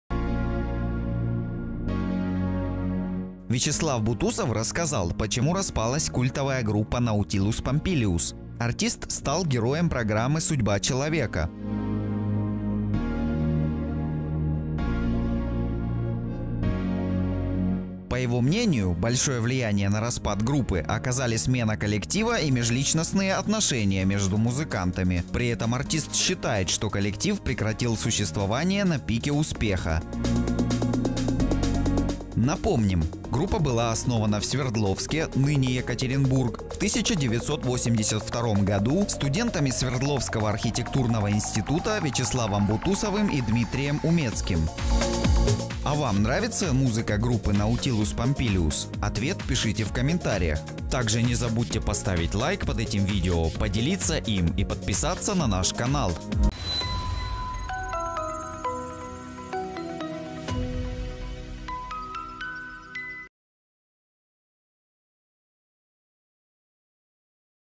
Бутусов озвучил причину распада группы «Наутилус Помпилиус» / RuNews24
Вячеслав Бутусов рассказал, почему распалась культовая группа «Наутилус Помпилиус».